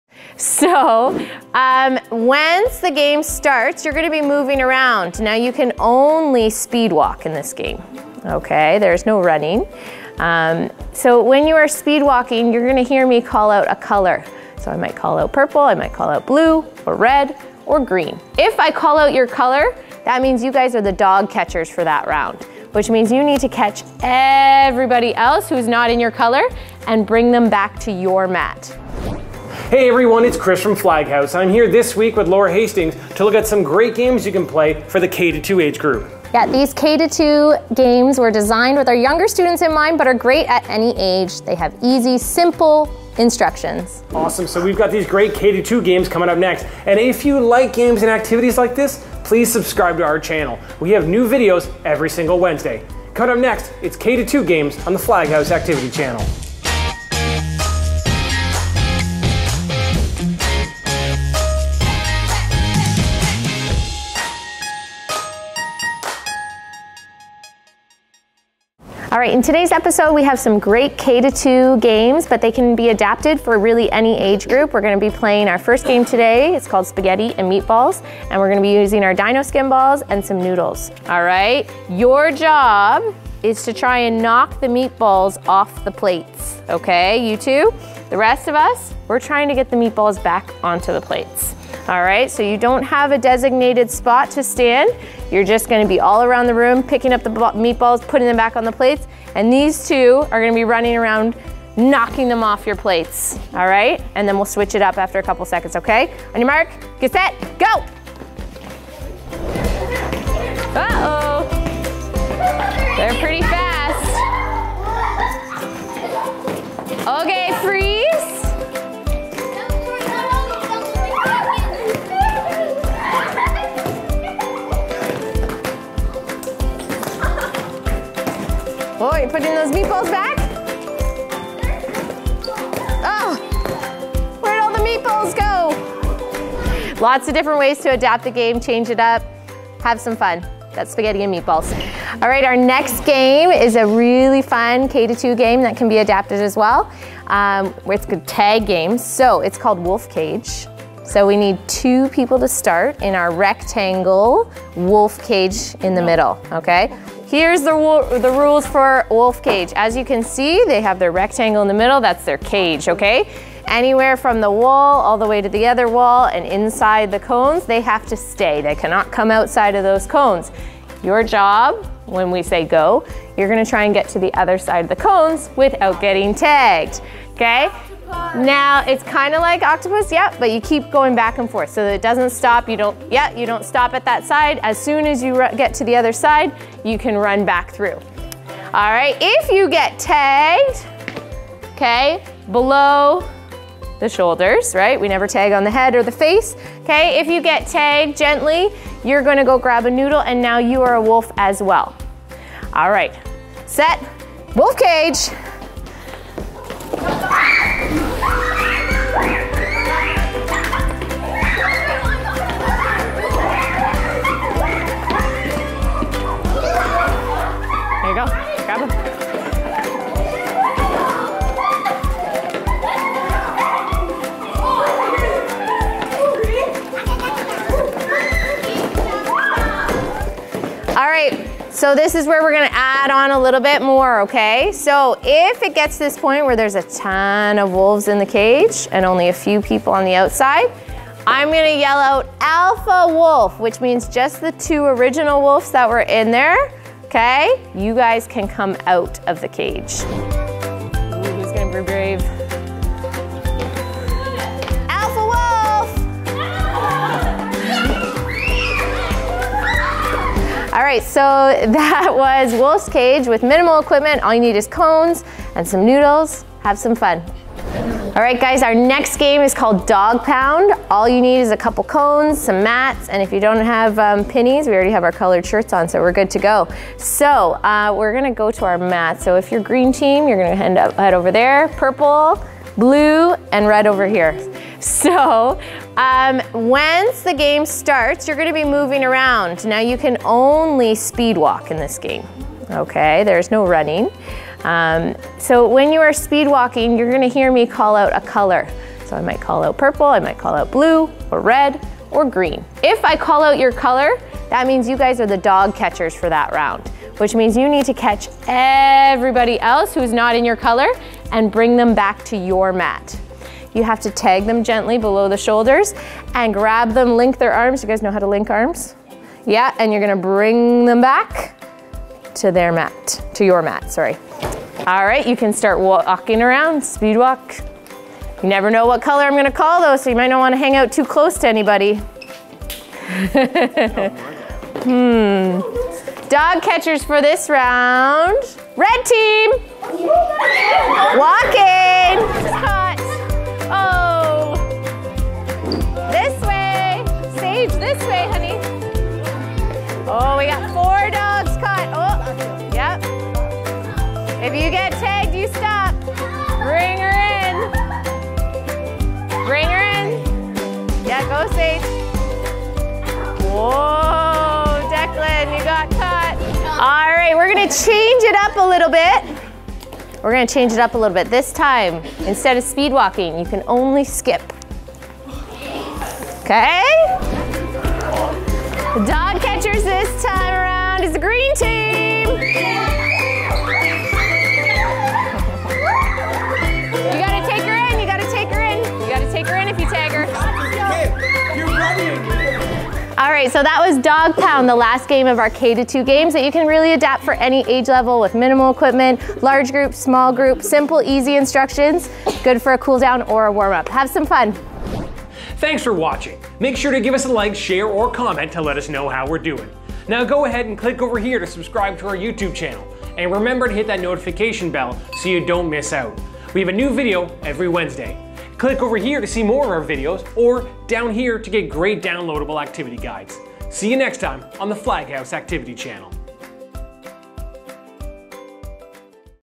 Phys Ed Ideas for Kindergarten to Grade 2 (Ep. 144 - K-2 Games)
Need to get a large group of young students engaged? Coach Laura has a great collection of games perfect for those kids that are small in size, but big on energy.

In this video:
Polyethylene - PE Rainbow Mat - 4 Side Hook and Loop Fasteners - 2' Panel - 4' x 6' - Item # 15365
Noodlecize Kit - Item # 19641
Plastic Hoops - 36'' - Dz - Item # 11021
Numbered Cones Set of 36 - Item # 18074
FlagHouse Mesh Pinnies Youth Size Super Set - Item # 14400